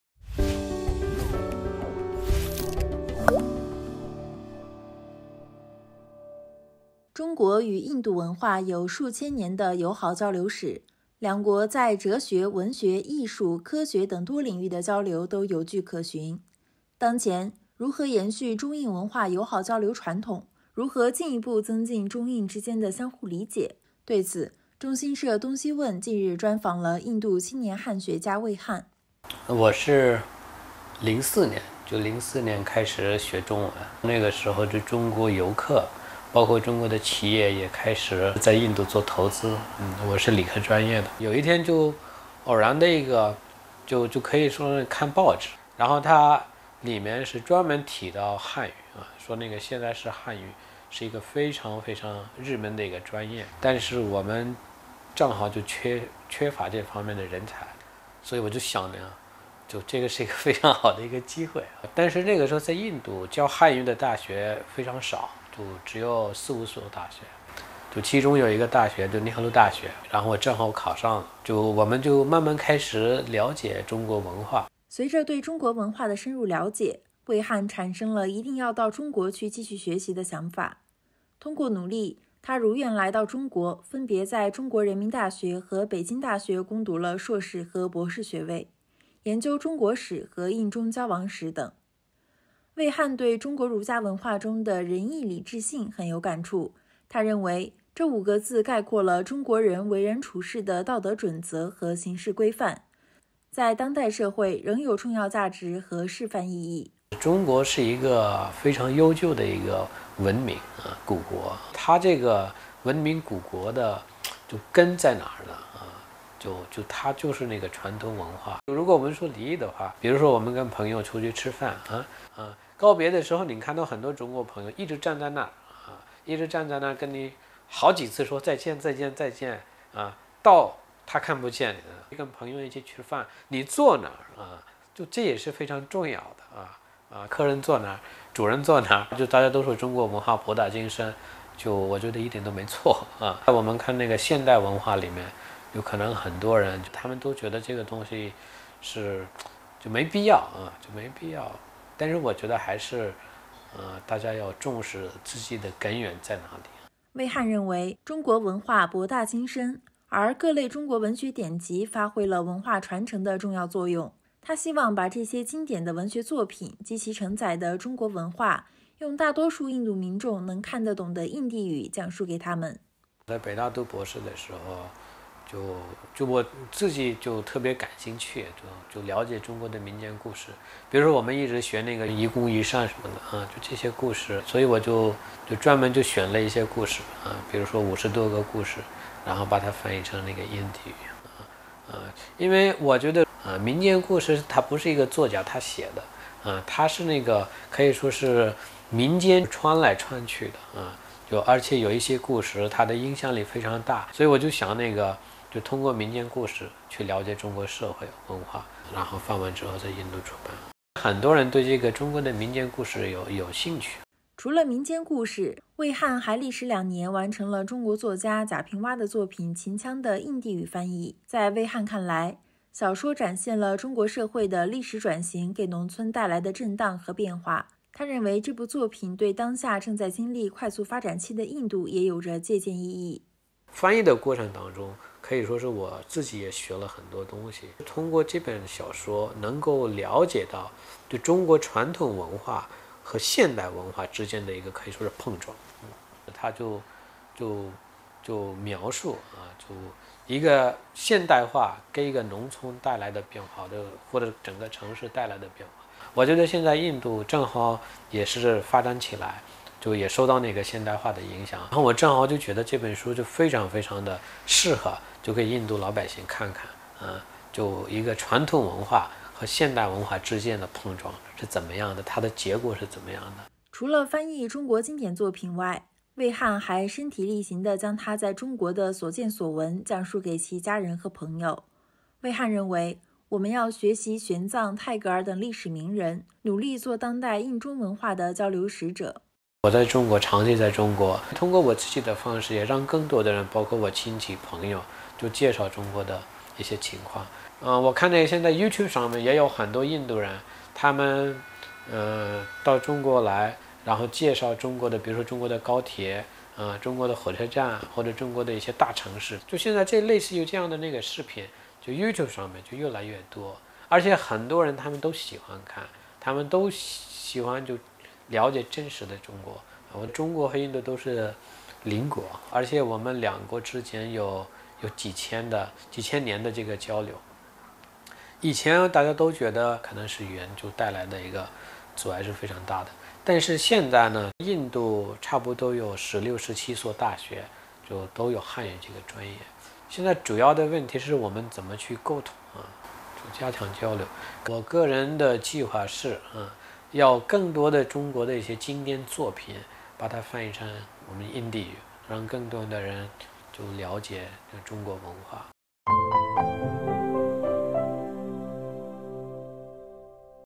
【东西问】印度青年汉学家魏汉：如何向印度民众讲述中国文化？
中国与印度文化有数千年的友好交流史，两国在哲学、文学、艺术、科学等多领域的交流都有据可循。当前，如何延续中印文化友好交流传统？如何进一步增进中印之间的相互理解？对此，中新社“东西问”近日专访了印度青年汉学家魏汉（VIKASH KUMAR SINGH）。